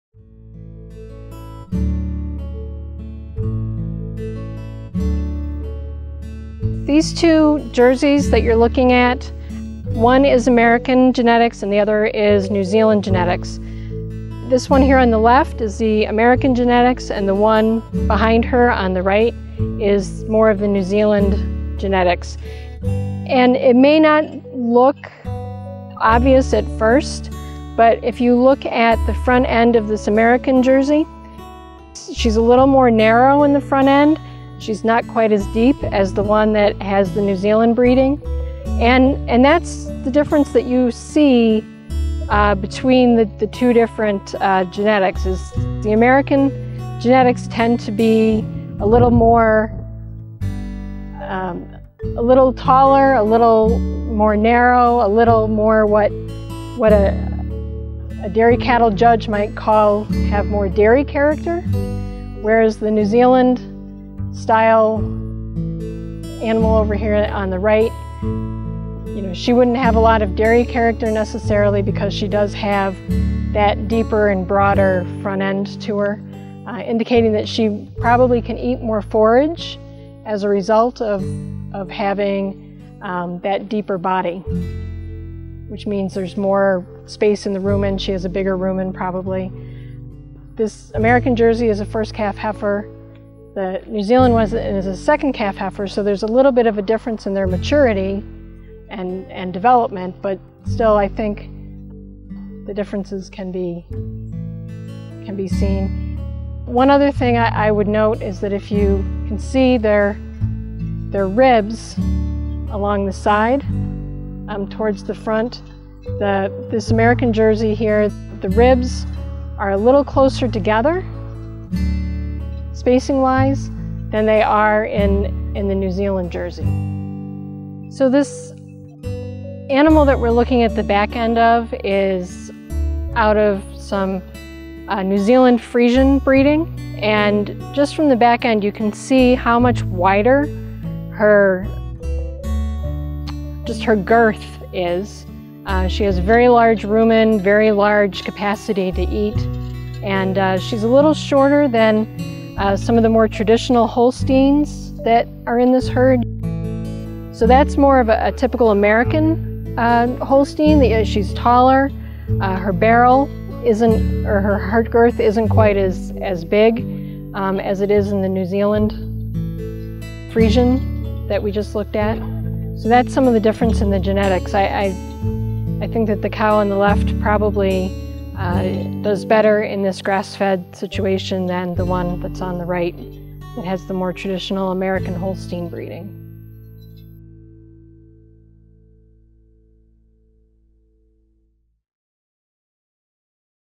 Grass Fed Dairy: American vs New Zealand Genetics
Karen Hoffman, USDA resource educator, explains the differences found between American and New Zealand based genetics in a Jersey breed.